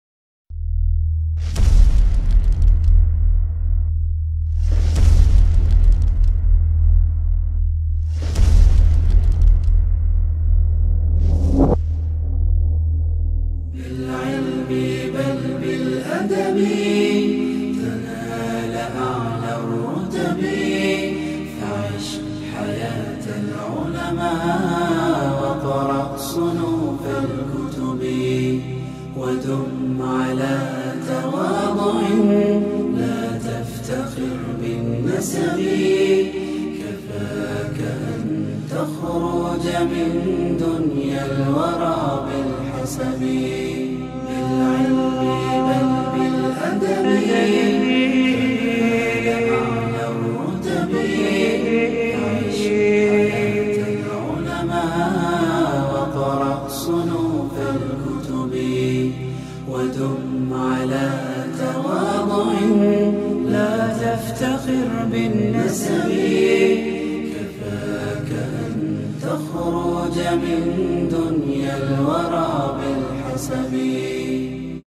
Daily Adhkar (invocations)
Daily Adhkar (invocations) Muslims say throughout the day.